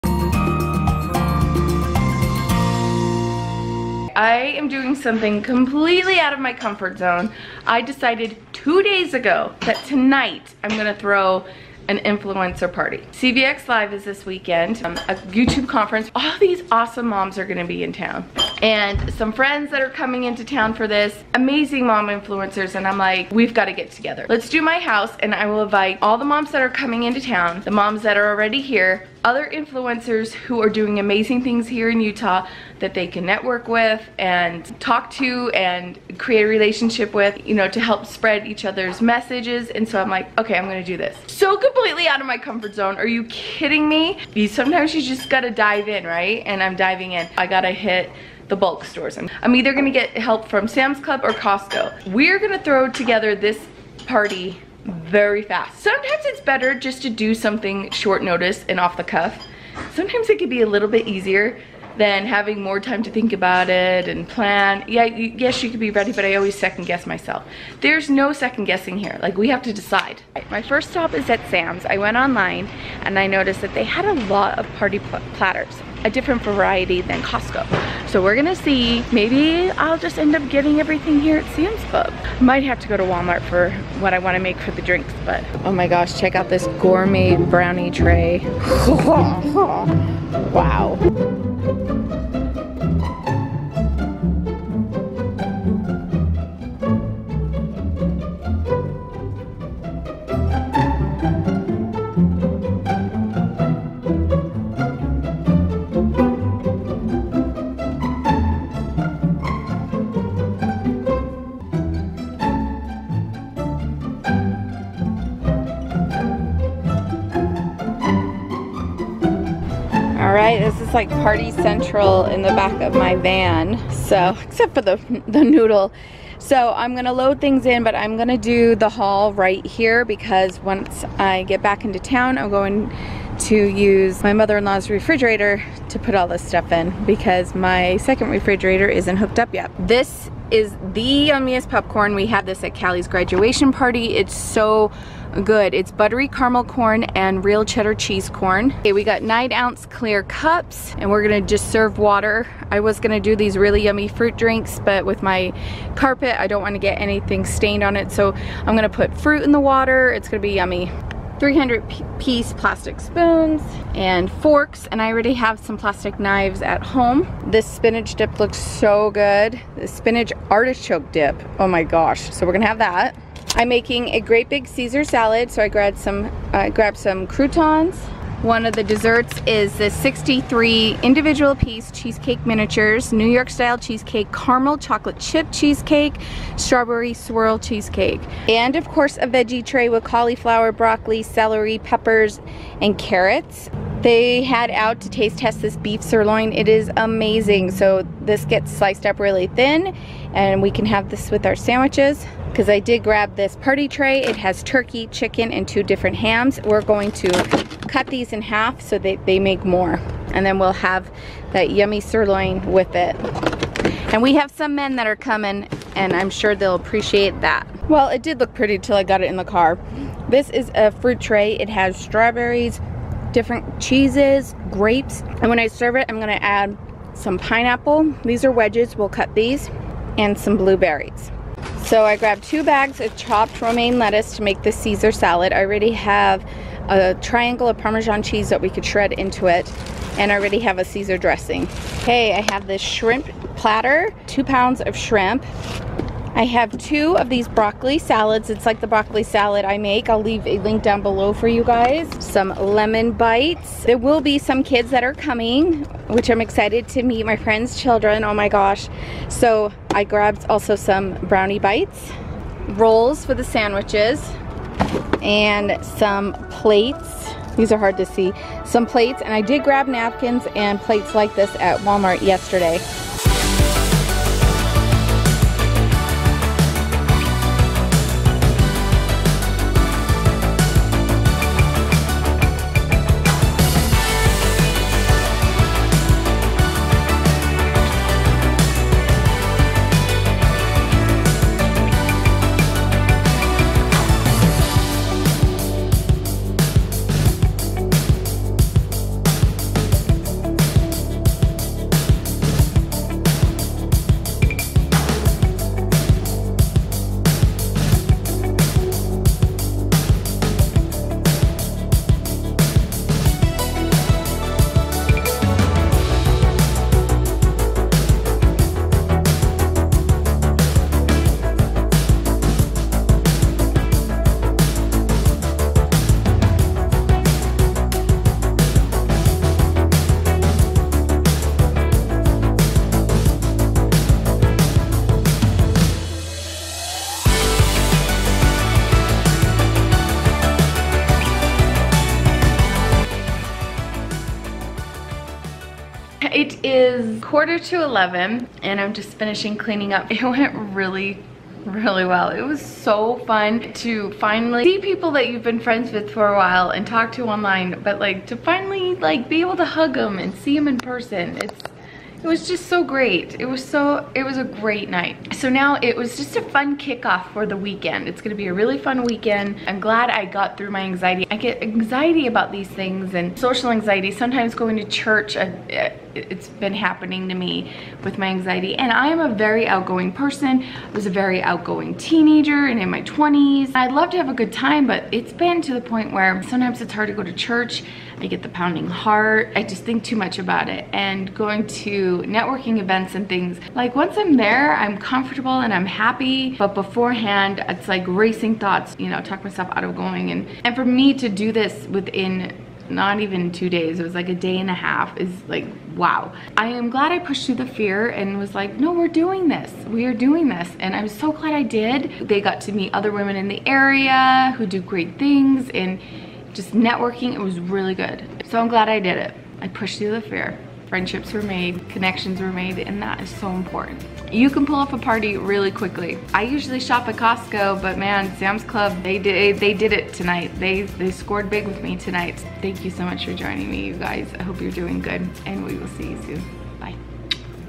Throwing A Last Minute Party- Sam's Club Haul
Throwing a last minute party can be done! Sometimes it can be just as easy to throw as the one you have planned for weeks. I'm getting help from Sam's Club.

The Tiny Home Movement
Made By Jen
Rebecca- soul healer